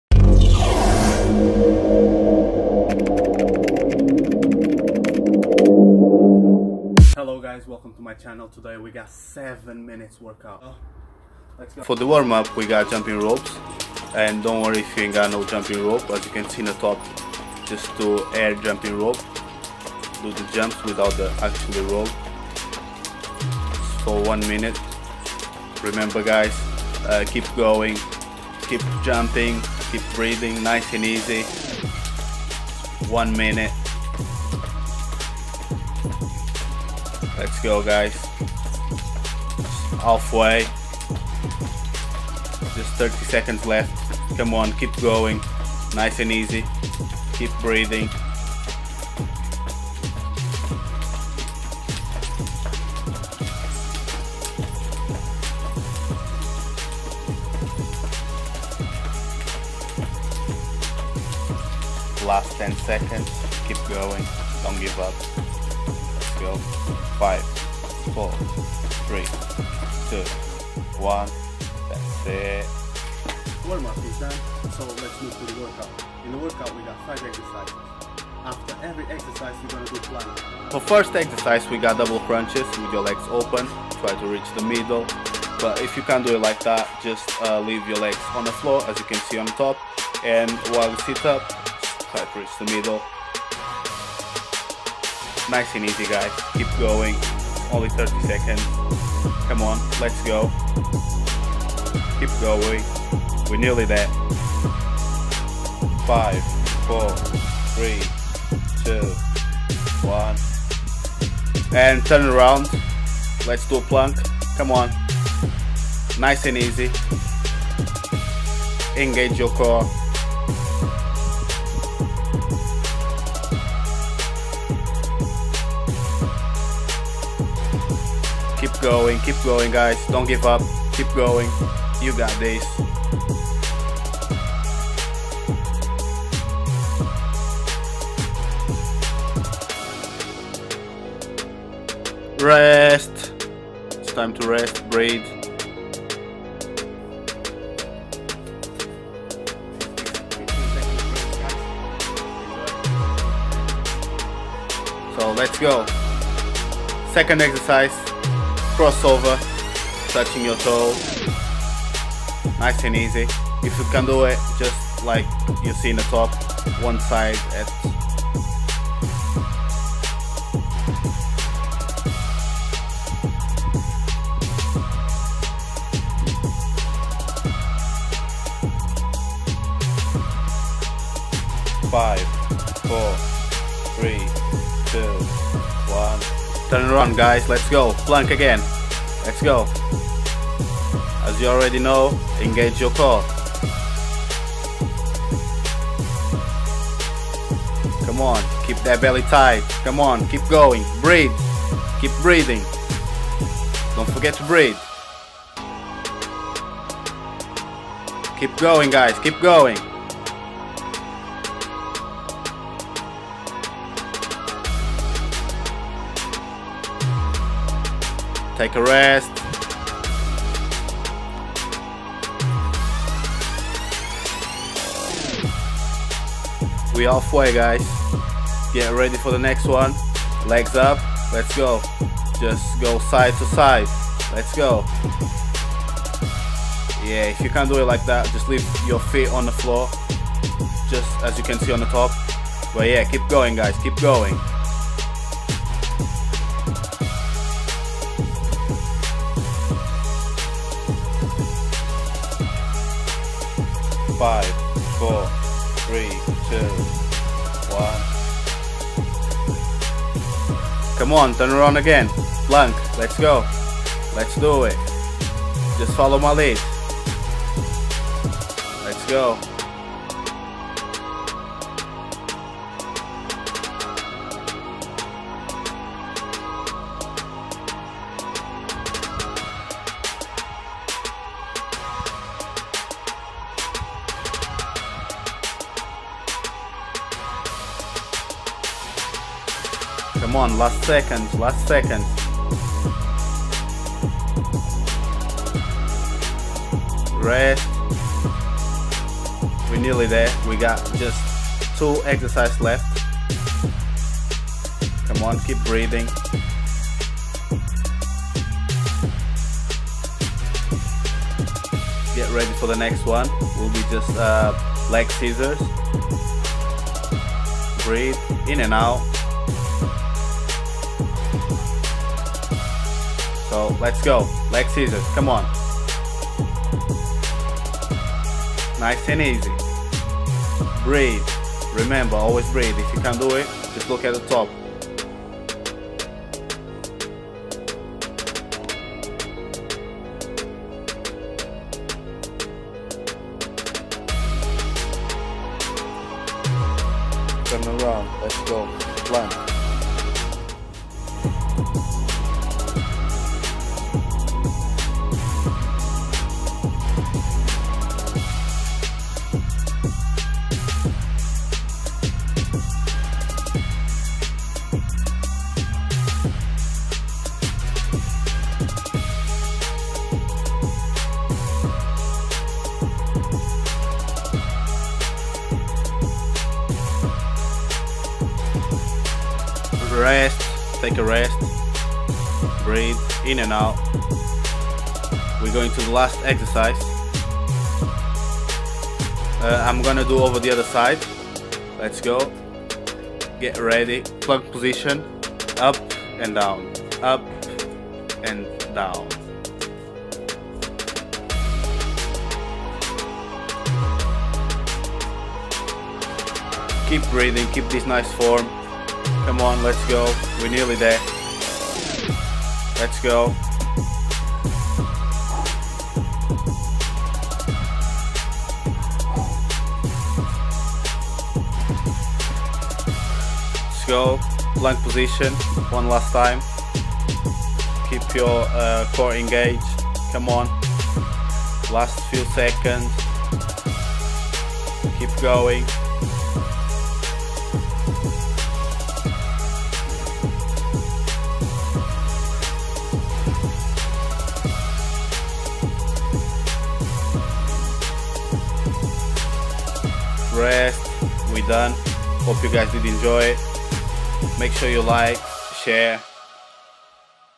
DKai 7 minutes Abs Killer
Be ready for this intense workout, only 7 minutes